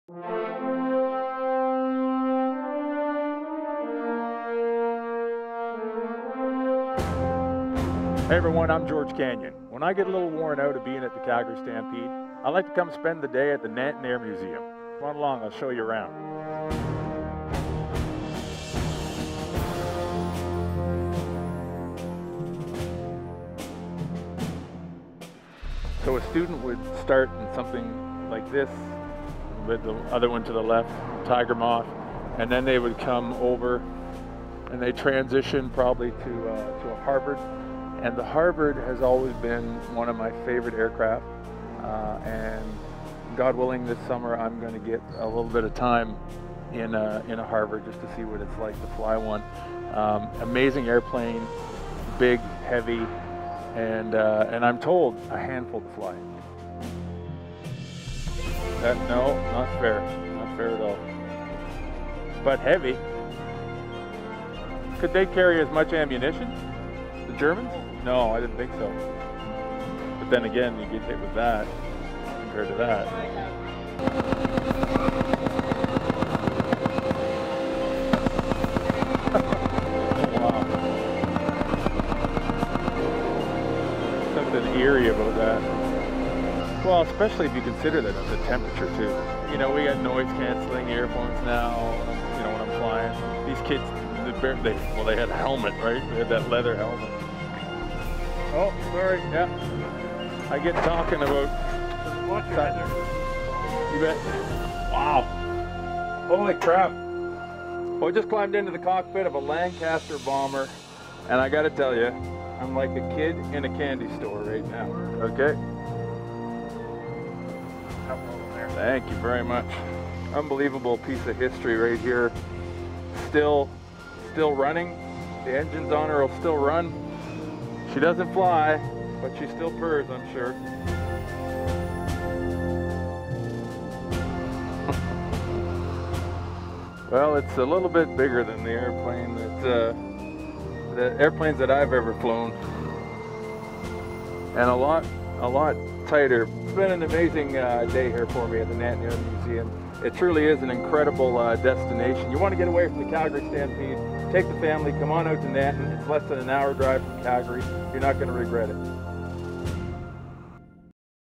George Canyon @ Nanton Air Museum
George Canyon shows us his favorite place to visit outside of the Stampede...The Nanton Air Museum.